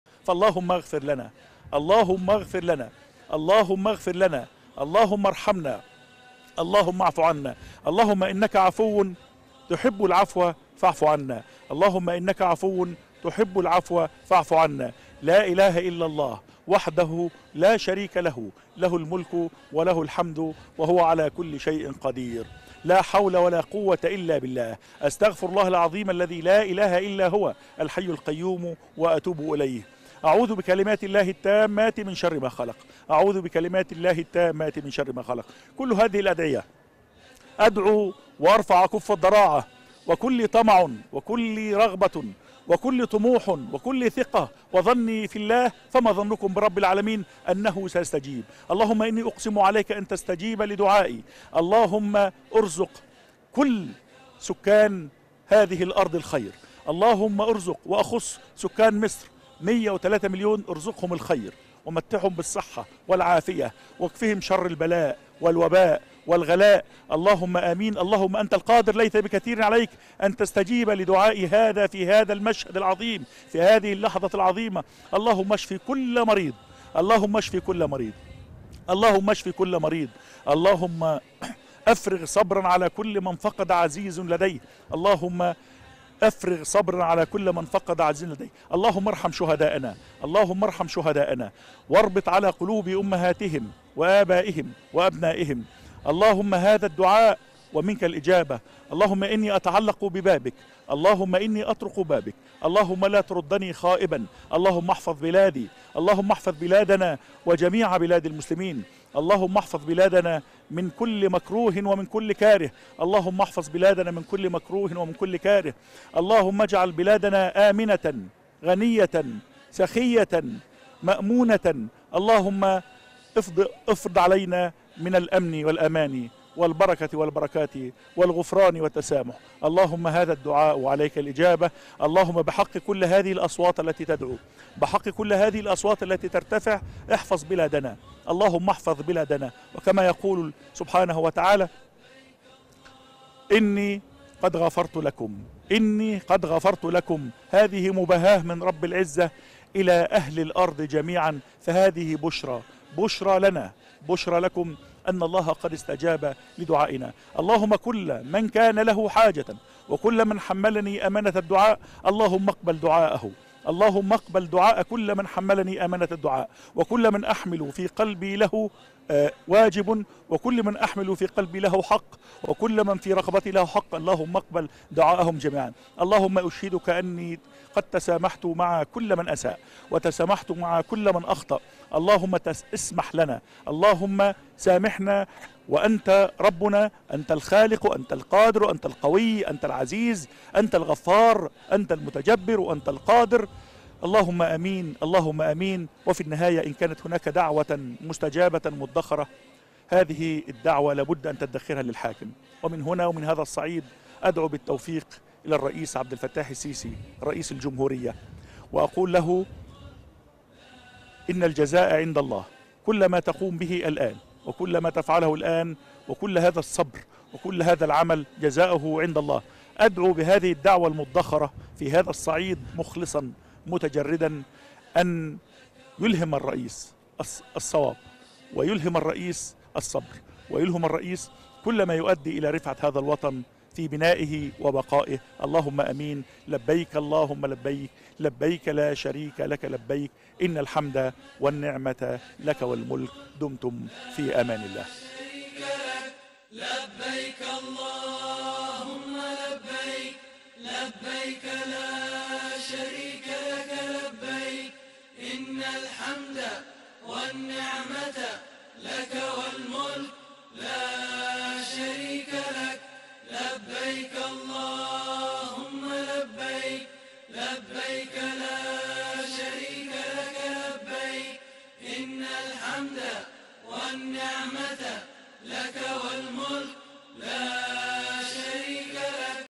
دعاء مؤثر من على جبل عرفات من الإعلامي نشأت الديهي
دعاء مؤثر من على جبل عرفات من الإعلامي نشأت الديهي حيث دعا للمصريين وللرئيس السيسي